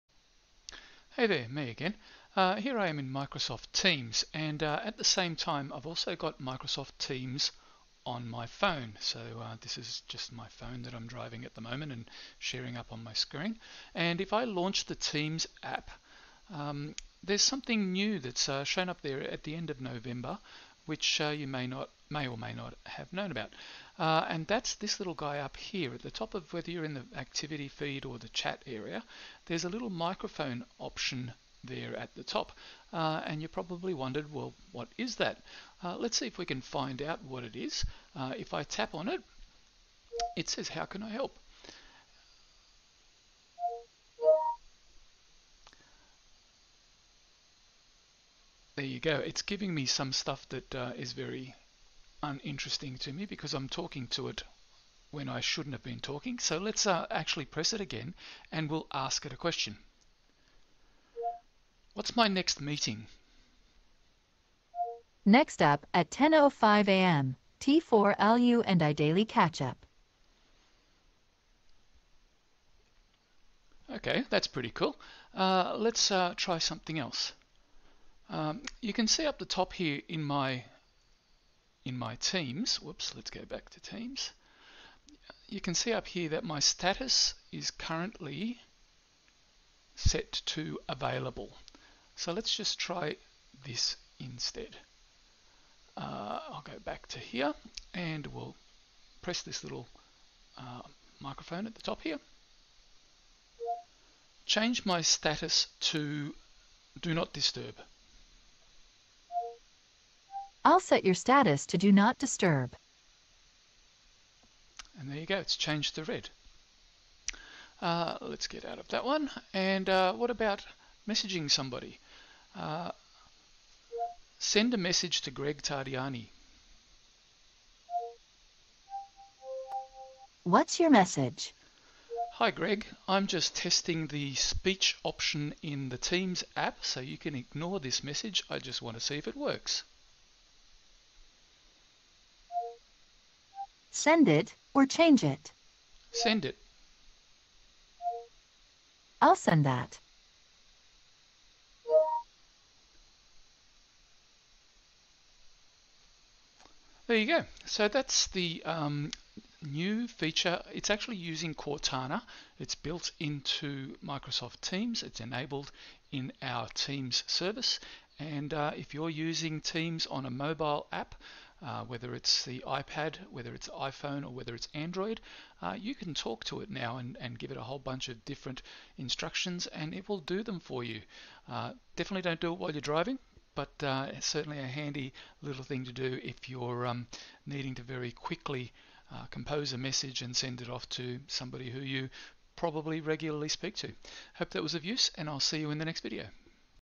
Voice control of the Teams app for Android or iPhone
In November 2021, Microsoft added Cortana features to the MS Teams app for Android and iPhone/iPad.  How does it work and what are some things you can do with it?  Watch this quick video for a demonstration.